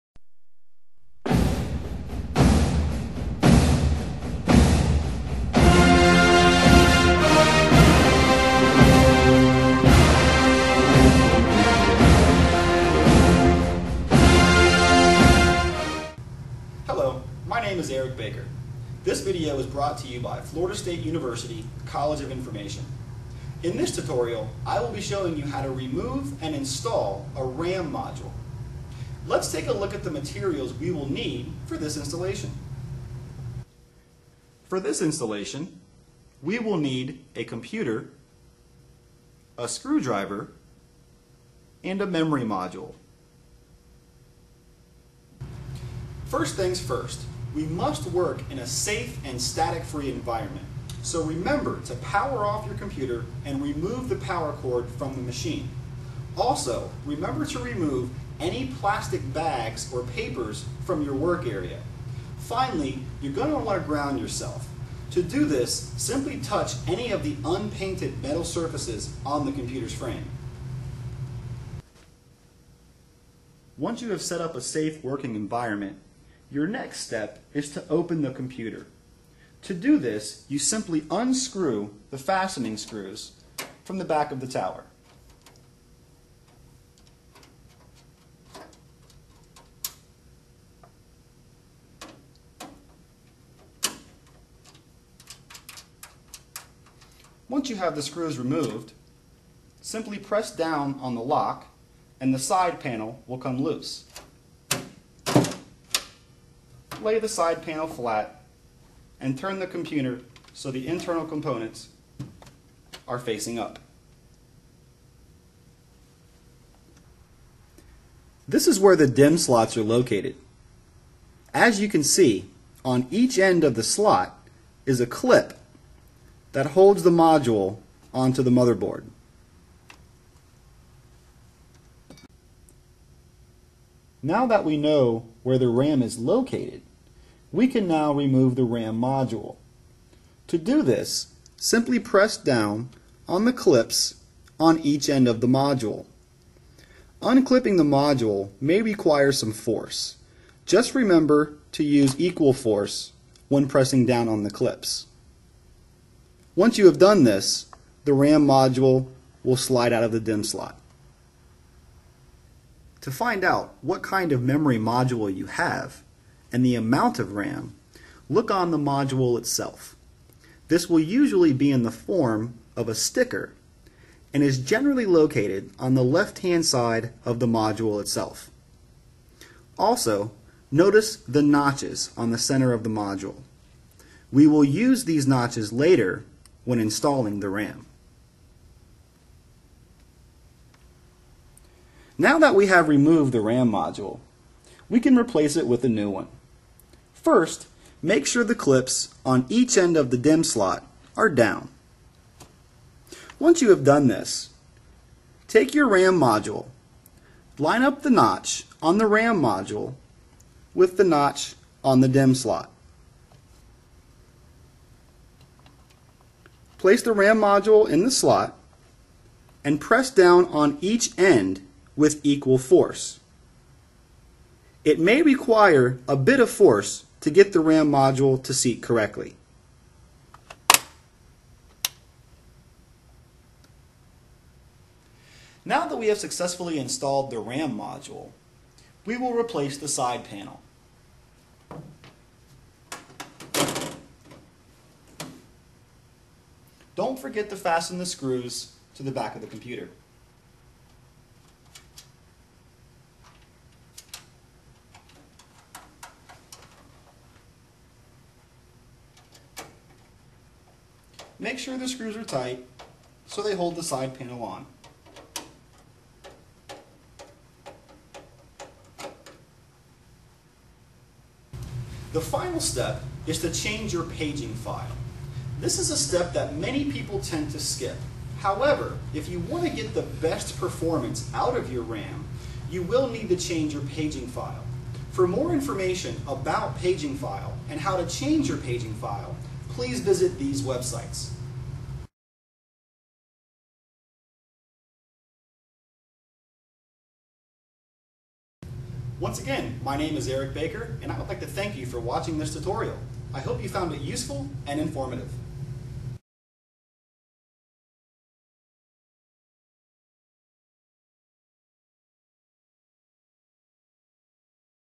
How to install and remove computer RAM
A basic computer hardware tutorial that covers how to properly remove and install a computer's memory, or Random Access Memory (RAM).

Produced for the  the Florida State University's College of Communication & Information and the LIS3353 Technologies course.

Created by Eric Baker Nathan Butterfield.

For more information about technology, Florida State, or the College of Communication and Information check out these links;